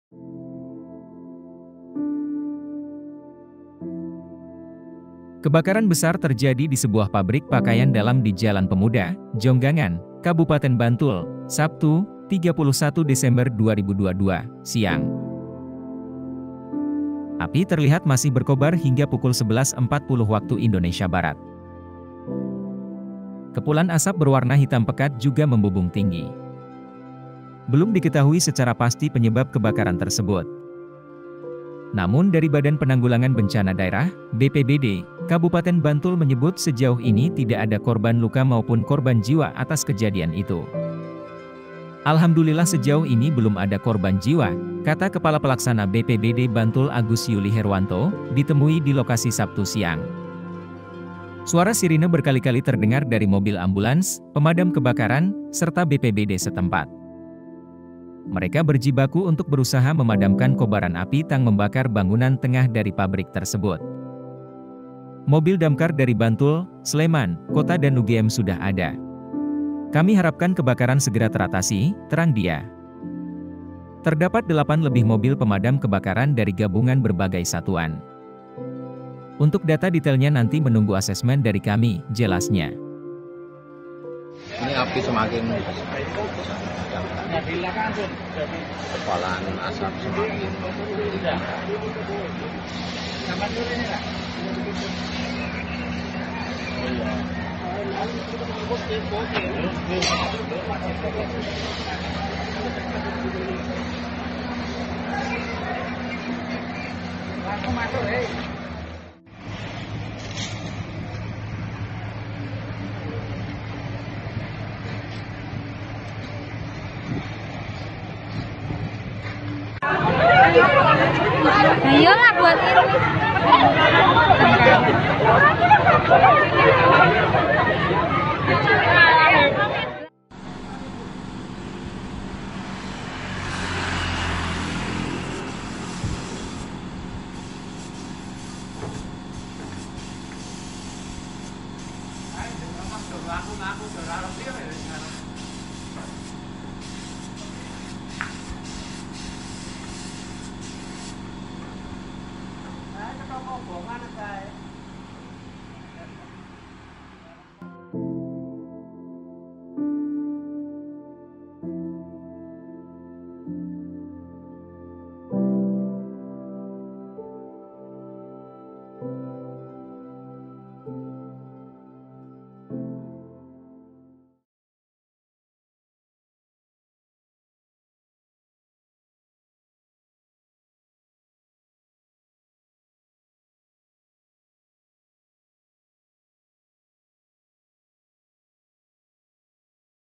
Kebakaran Hebat Hanguskan Pabrik Pakaian Dalam di Bantul, Api Membesar dan Asap Hitam Mengepul
POS-KUPANG.COM - Kebakaran besar terjadi di sebuah pabrik pakaian dalam di Jalan Pemuda, Jonggangan, Kabupaten Bantul, Sabtu (31/12/2022) siang. (POS-KUPANG.COM, Miftahul Huda)

Artikel ini telah tayang di TribunJogja.com dengan judul Kebakaran Hebat Hanguskan Pabrik Pakaian Dalam di Bantul, Api Membesar dan Asap Hitam Mengepul,

Penulis  : Miftahul Huda
Editor  : Kurniatul Hidayah
Video Editor : Leo R